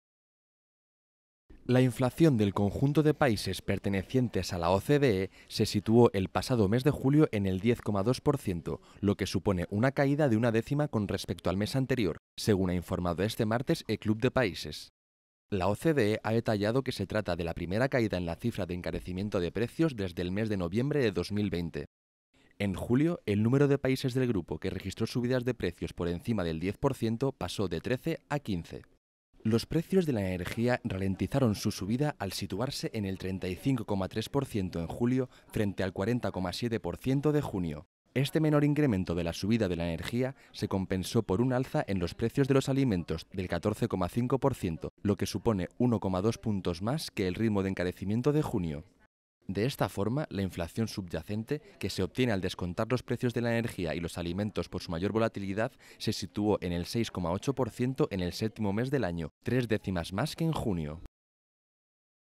La inflación de la OCDE baja por primera vez desde noviembre de 2020
La inflación del conjunto de países pertenecientes a la Organización para la Cooperación y el Desarrollo Económicos (OCDE) se situó el pasado mes de julio en el 10,2%, lo que supone una caída de una décima con respecto al mes anterior, según ha informado este martes el club de países.

La OCDE ha detallado que se trata de la primera caída en la cifra de encarecimiento de precios desde el mes de noviembre de 2020. En julio, el número de países del grupo que registró subidas de precios de doble dígito pasó de 13 a 15.

(Fuente: Imágenes Cedidas)